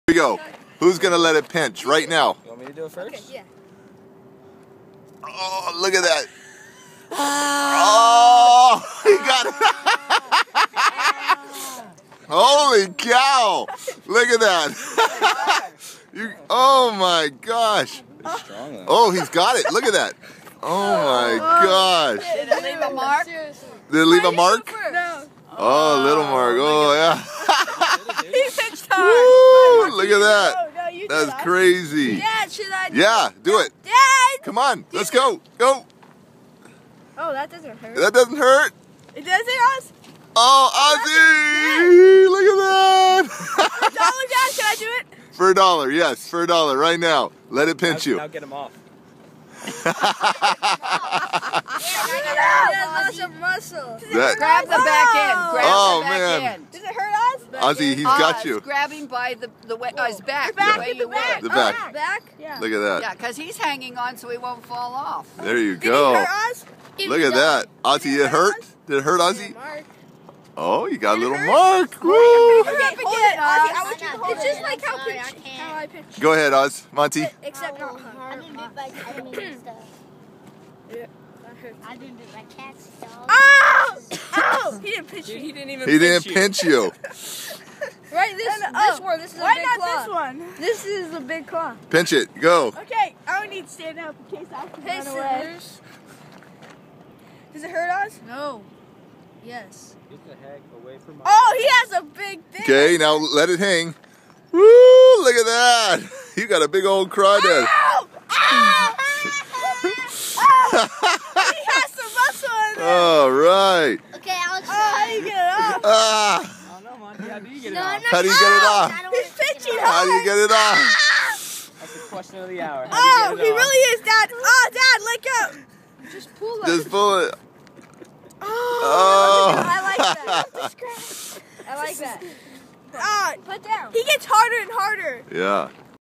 Boys Get Pinched By A Giant Crawdad
Derek Clark's boys find a giant crawdad and they get pinched.